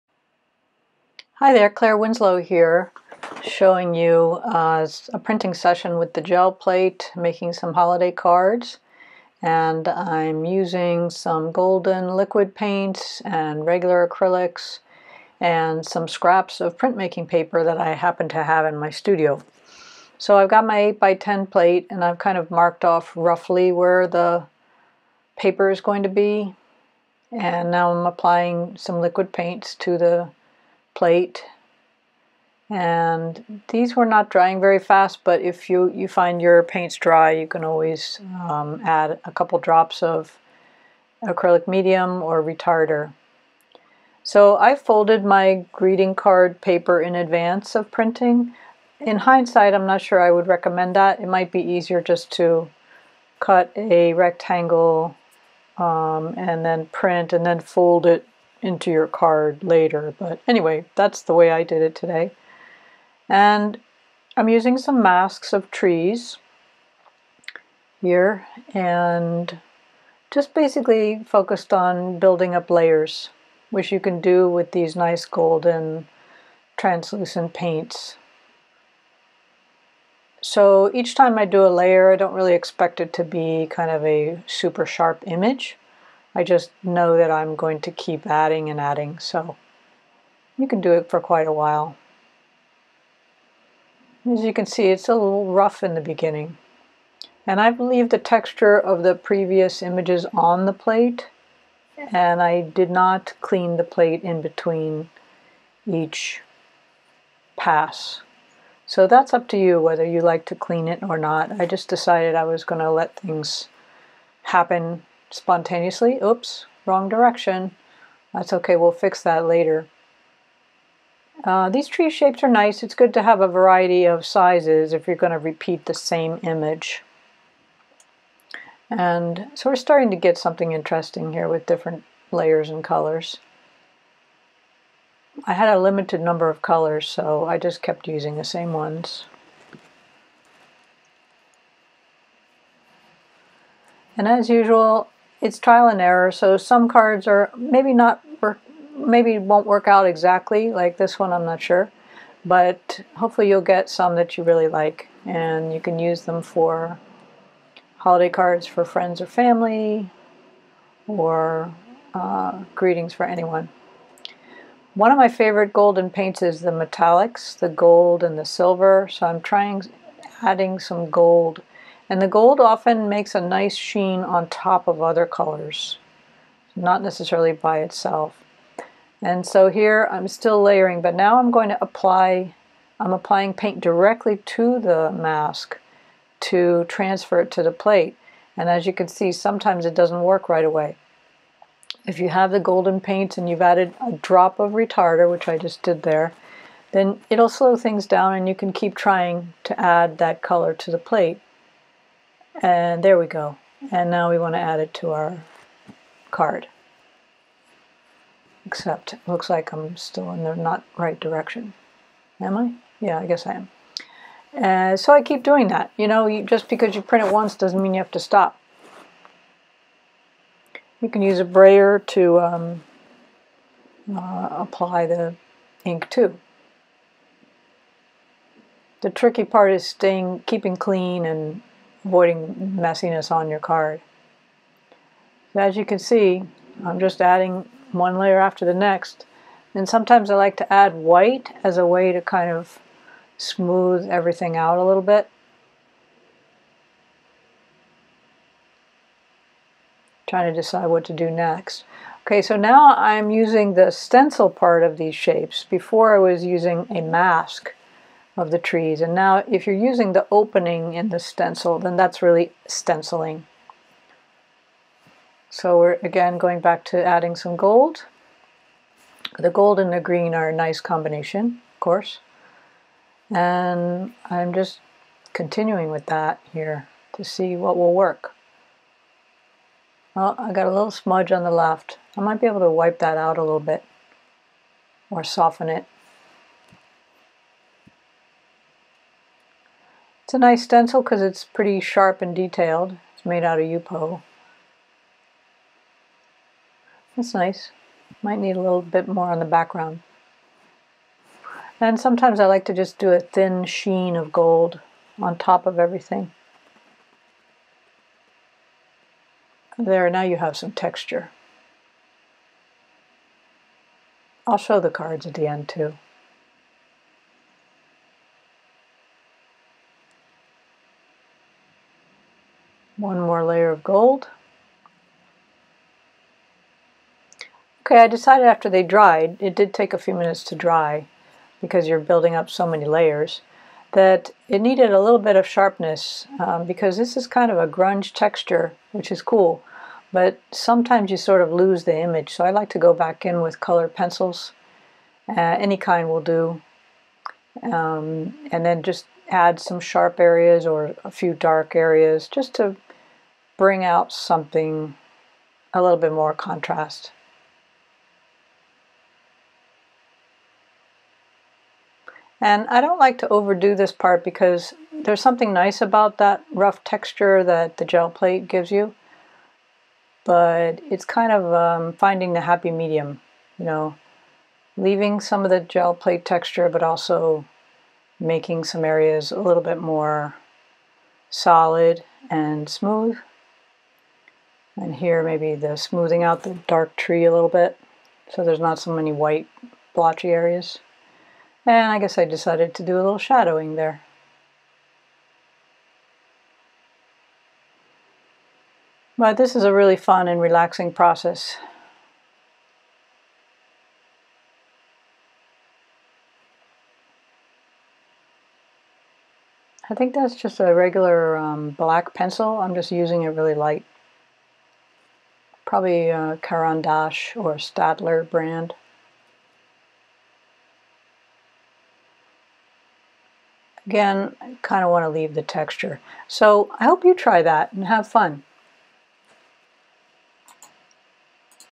Holiday Cards with the Gel Plate
This video shows you one way to make holiday cards with the gel plate using gel printing with Golden translucent paints and stencils. A 7-minute video with audio.

MATERIALS
Golden Acrylic paints, regular body and liquid
Smooth heavyweight paper (not printer/copy paper).
Printmaking paper such as Arches is good, or smooth watercolor paper.
Brayer/roller
gelatin plate
stencil/mask